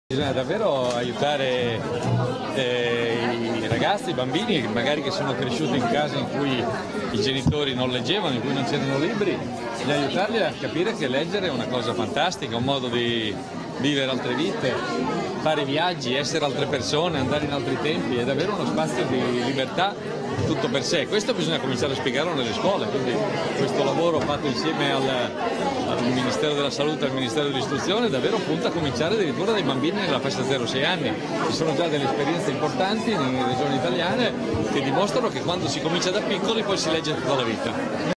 Franceschini " Aiutare i bambini a leggere "
Franceschini " Aiutare i bambini a leggere " Così il Ministro alla conferenza " Cultura, Scuola e Salute "

Èliveromatv​ la Capitale Cambia Canale CANALE 14